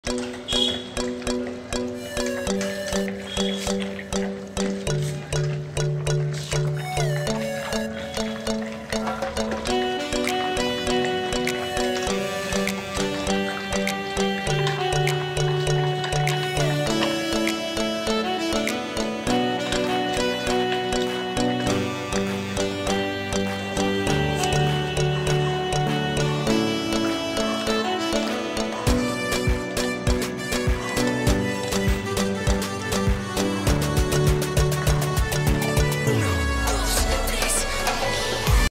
broogon || eating their food || st.bernard' broofy & dragon
A 60-70kg st.bernard eat 3 tym in a day normally.
I give my dogs food i.e. royal canine / drools with  milk as per meal.